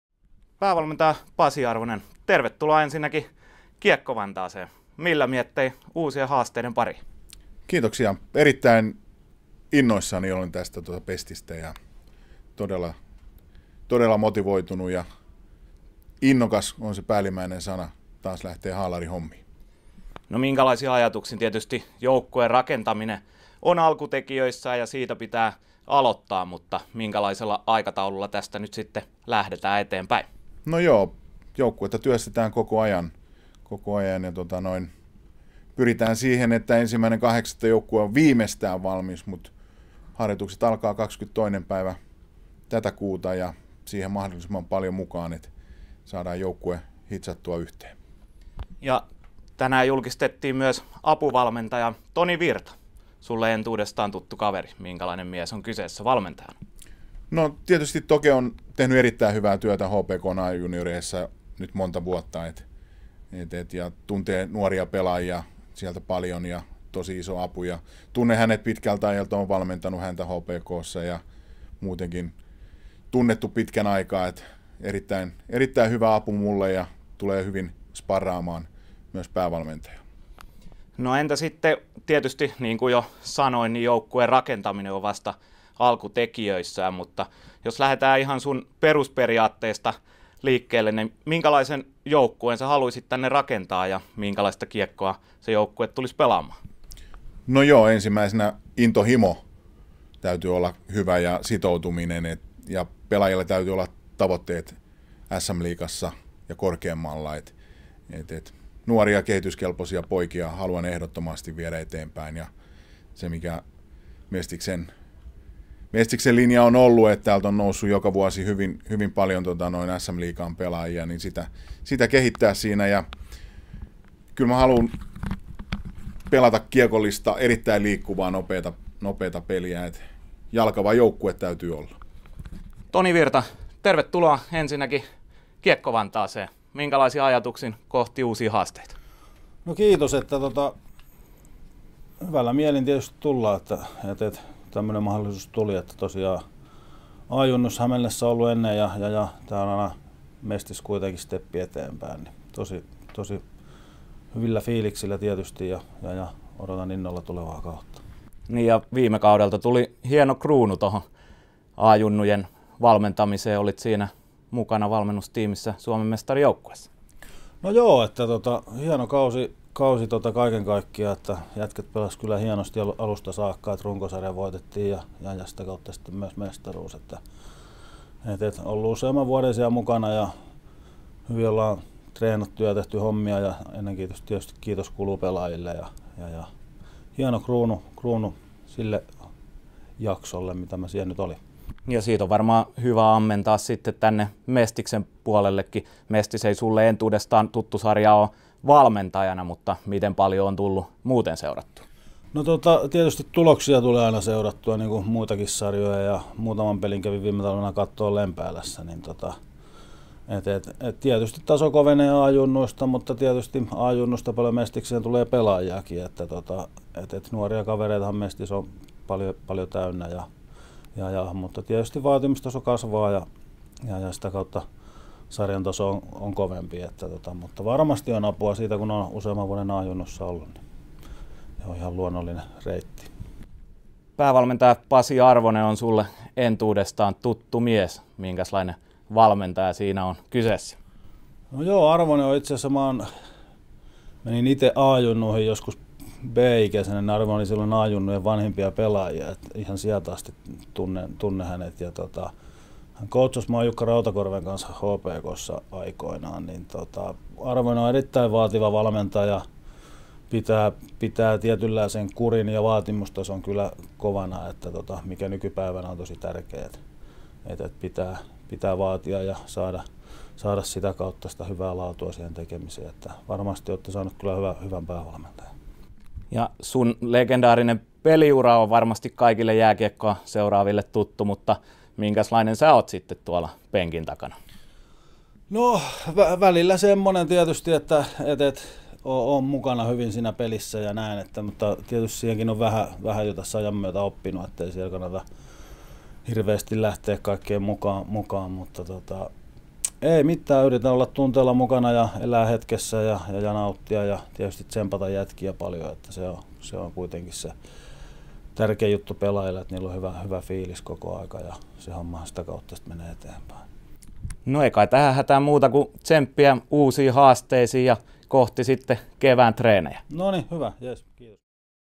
Kauden 2017-2018 valmennuskaksikko haastattelussa
Kiekko-Vantaan kauden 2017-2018 valmennuskaksikko Pasi Arvonen ja Tony Virta haastattelussa.